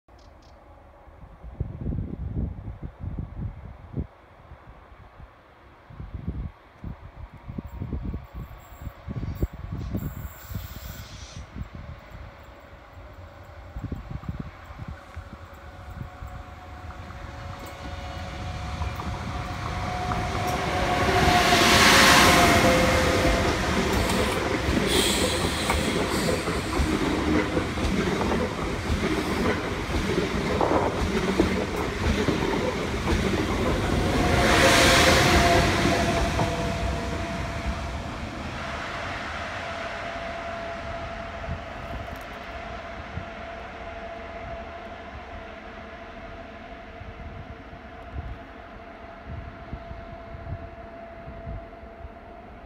Tren Alvia S-130 decoración 75 ANIVERSARIO por el túnel de Elda.
Tren Alvia S-130 (Euromed 01101) con decoración RENFE 75 aniversario procedente de Barcelona pasando por el túnel de Elda lentamente con destino Alicante.
Grabado el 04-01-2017 a las 14:30.